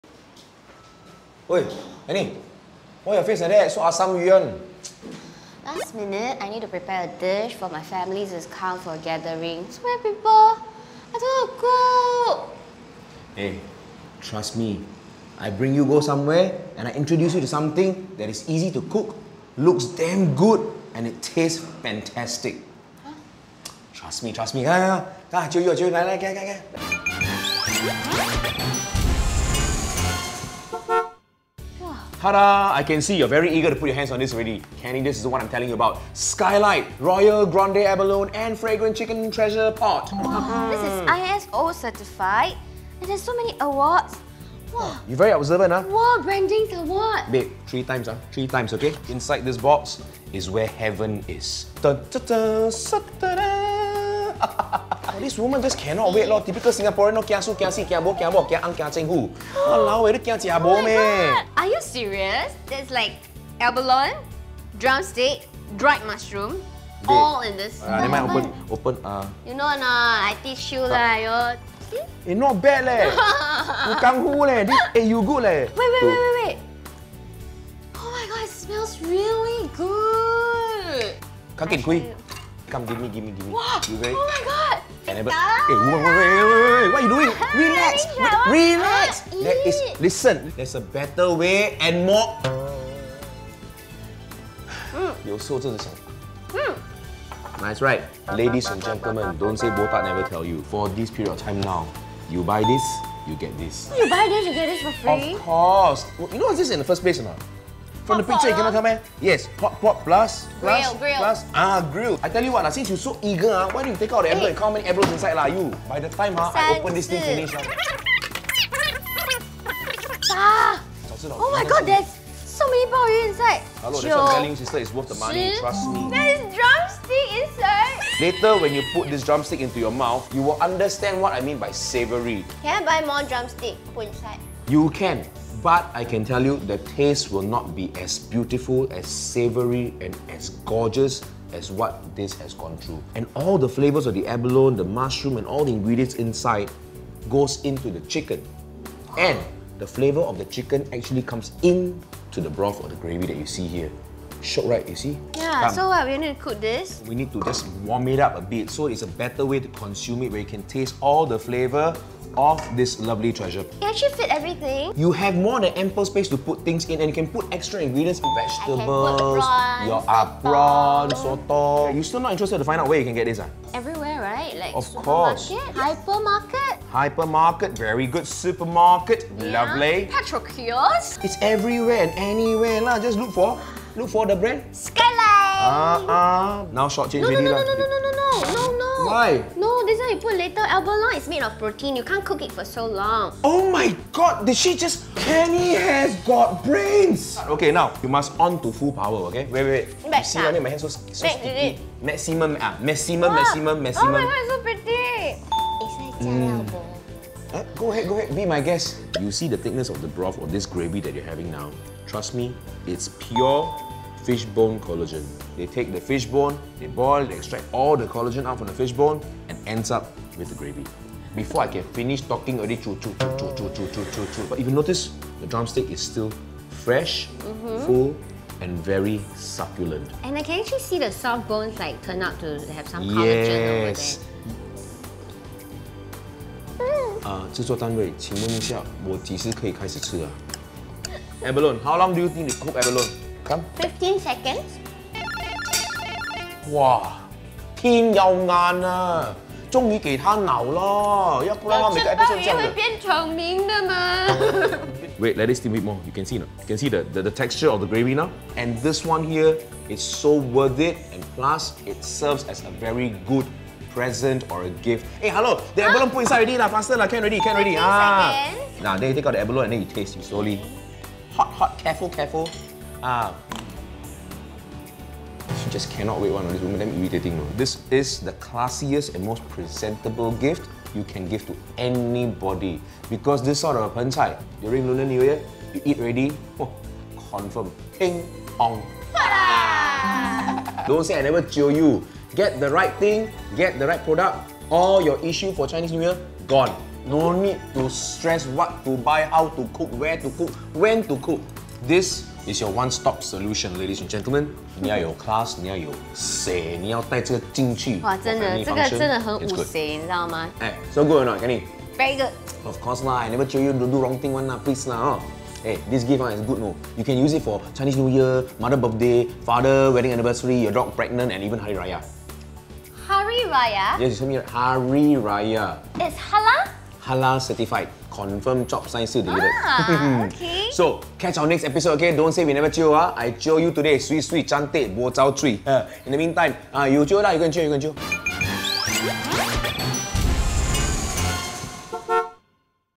Jio U Go Skylight Treasure Pot
Botak Jio U how to serve a treasure pot (盆菜) for your reunion dinner.

Get the Skylight Royal Grande Abalone & Fragrant Chicken Treasure Pot and receive a FREE Multi-purpose Electric Hot Pot.

Available at All major Petrol Stations, Supermarkets and Hypermarkets.